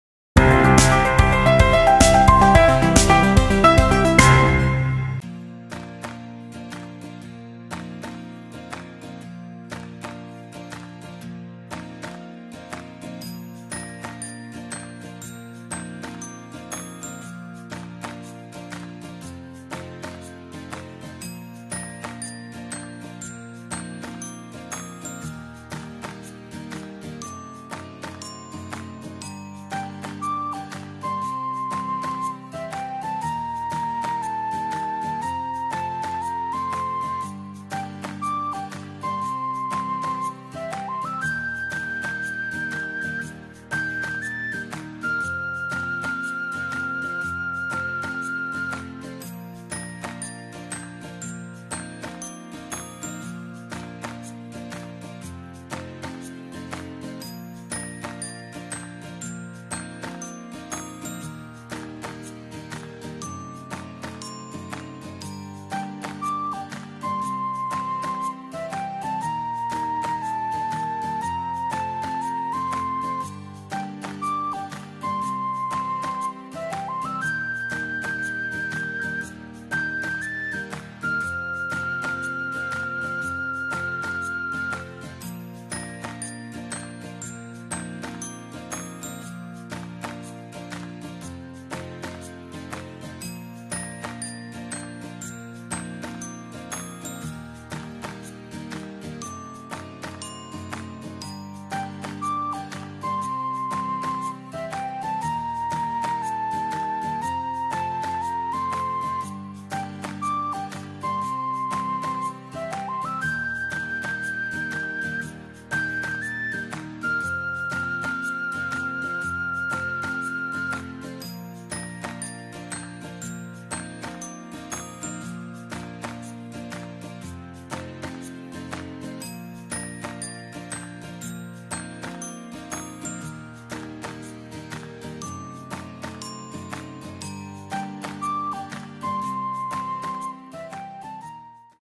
What's Inside at NAIA Terminal 1 (Old Domestic Airport) | What to Eat and Drink
Watch this video on what's inside at NAIA Terminal 1
Where to Eat and Drink